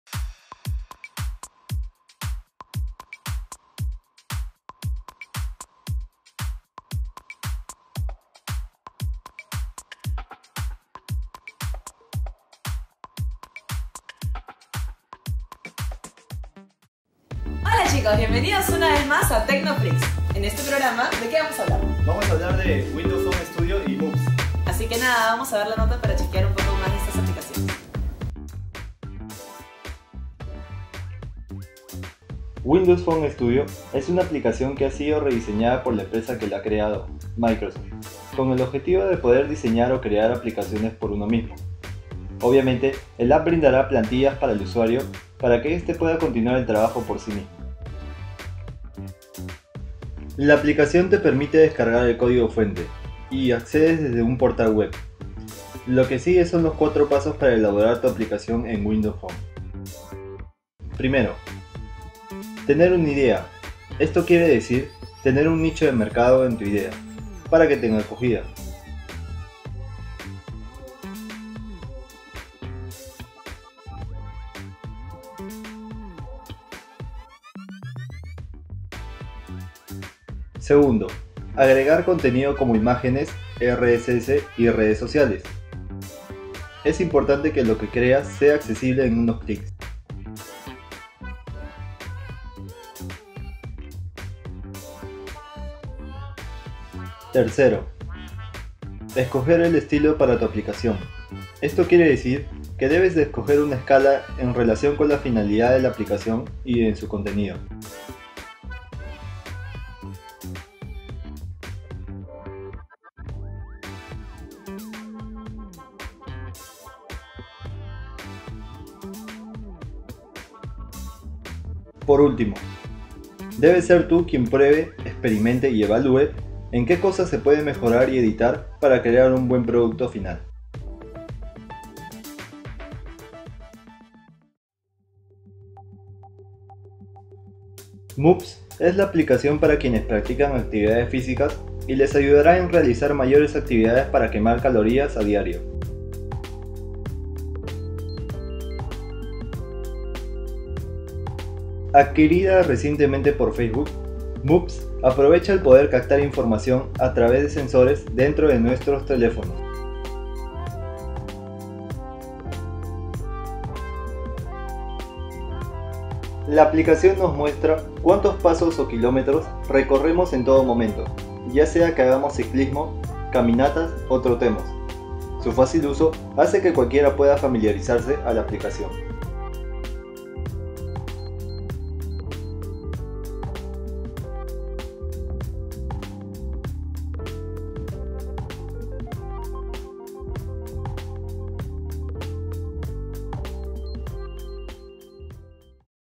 Tecnofreaks | Windows Phone Studio y Moves
En esta edición de tecnofreaks hablaremos sobre "Windows Phone Studio" y "Moves", interesantes aplicaciones nos puede ser útil en todo momento, veamos la nota.